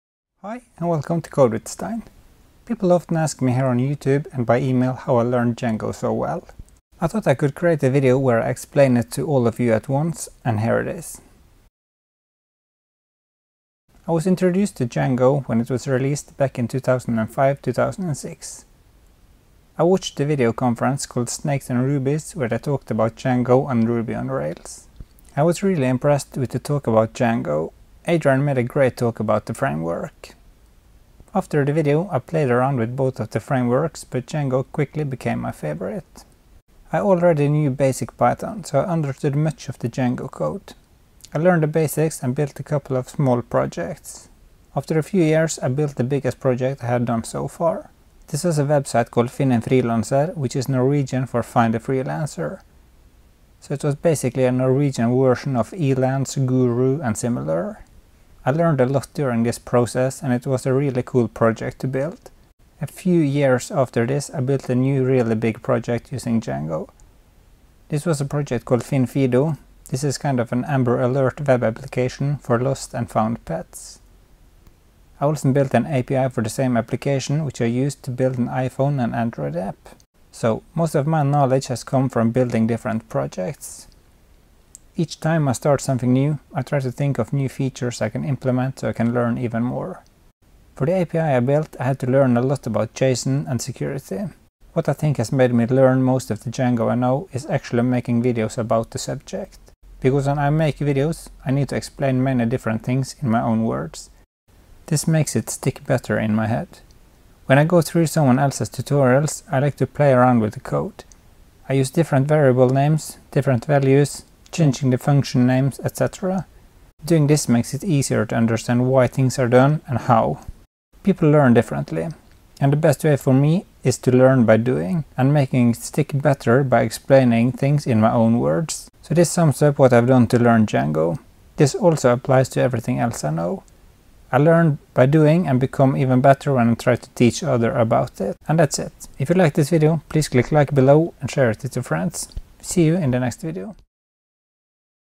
How I learned Django so well
People often ask me here on YouTube and by e-mail how I have learned Django so well. I thought I could create a video where I explain it to all of you at once. and here it is.

I was introduced to Django when it was released back in around 2005/2006. I watched a video conference called Snakes and Rubies where they talked about Django and Ruby on Rails. I was really impressed with the talk about Django. Adrian made a great talk about the framework.

After the video, I played around with both of the frameworks, but Django quickly became my favorite. I already knew basic Python, so I understood much of the Django code.

I learned the basics and built a couple of small projects. After a few years, I built the biggest project I had done so far. This was a website called "finn en frilanser" which is Norwegian for "find a freelancer". So it was basically a Norwegian version of elance, guru and similar. I learned a lot during this process and it was a really cool project to build.

A few years after this, I built a new really big project using Django. This was a project called "FinnFido". This is kind of an amber alert web application for lost and found pets. I also built an API for the same application which I used for an iPhone and Android app.

So, most of why knowledge has comes from building different projects. Each time I start something new, I try to think of new features I can implement so I can learn even more. For the API I built, I had to learn a lot about JSON and security.

What I think has made me learn most of the Django I know, is actually making videos about the subject. Because when I make videos, I need to explain many different things in my own words. This make it stick better in my head.

When I go through someone elses tutorials, I like to play around with code. I use different variable names and values, change the function names etc. Doing this makes it easier to understand why things are done the way they are.

People learn differently, and the best way for me to learn is "learn by doing" and making it stick better by explaining things in my own words. So this sums up what I have done to learn Django.

This also applies to everything else I know. I learn by doing and becomes even better when I try to teach other about it.